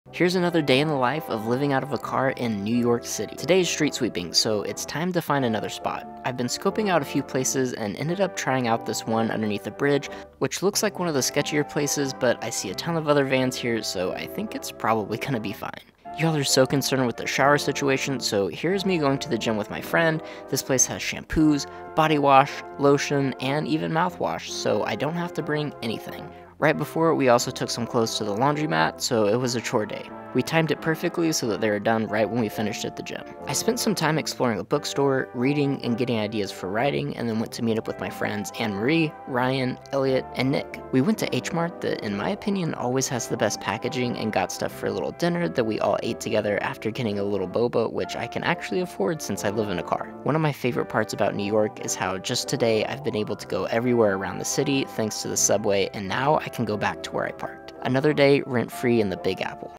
Living out of a car in New York City
I've been spending some time living in the big apple out of my car. Here is a little daily vlog to show what life is like.